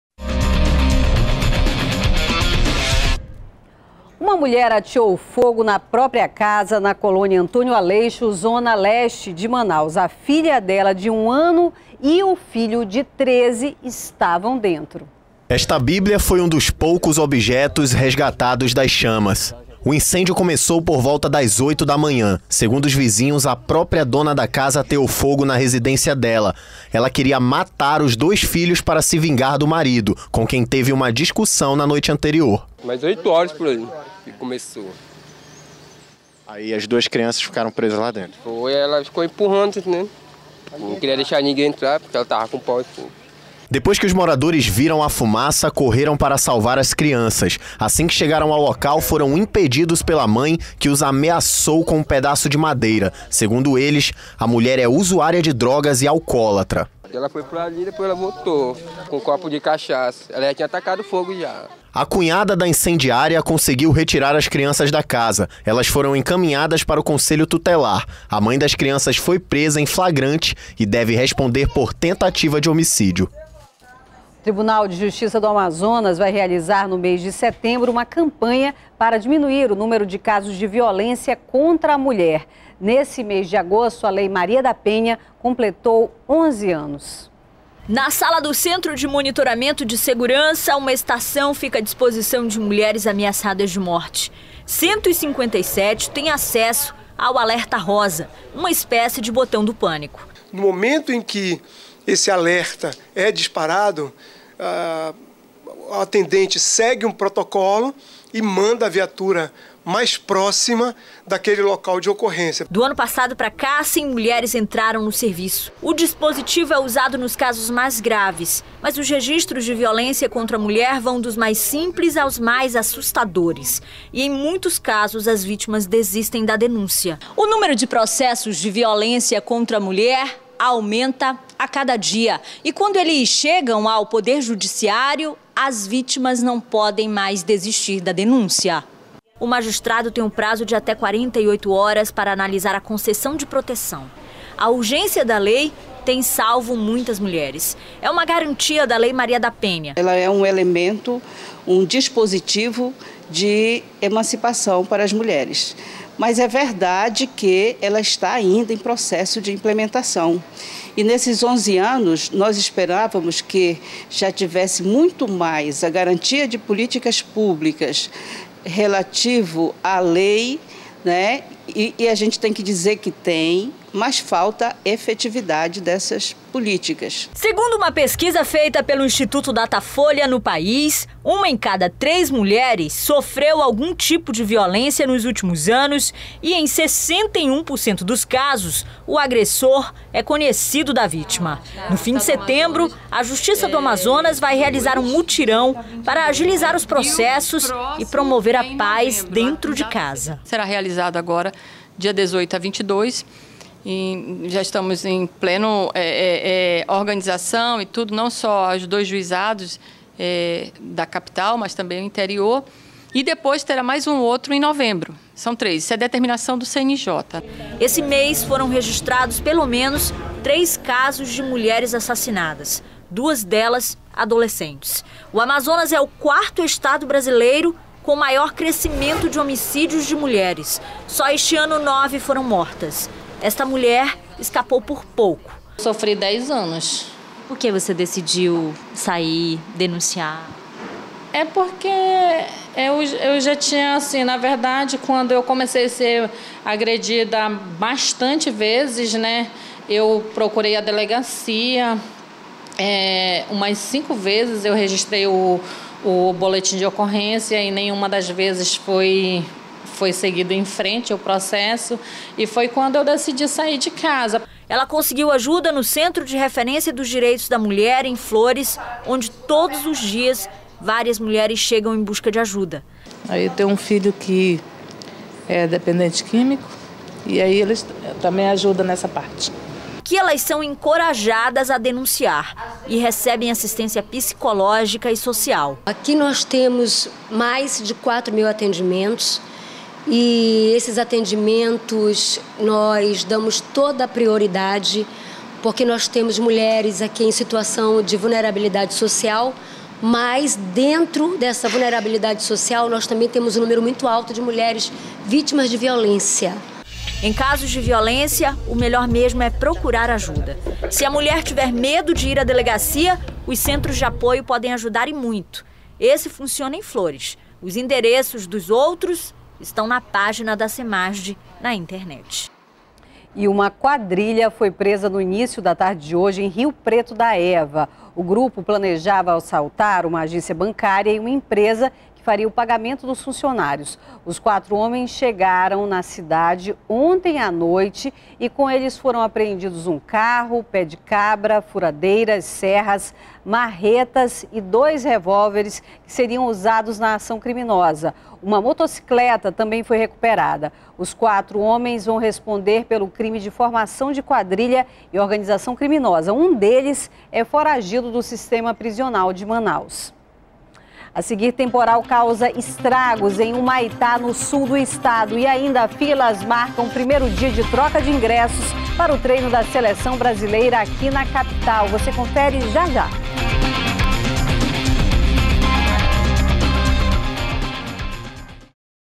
Mulher incendeia a própria casa com os dois filhos dentro na Colônia Antônio Aleixo
02º BLOCO JORNAL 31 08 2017 - Mulher incendeia a própria casa com os dois filhos dentro na Colônia Antônio Aleixo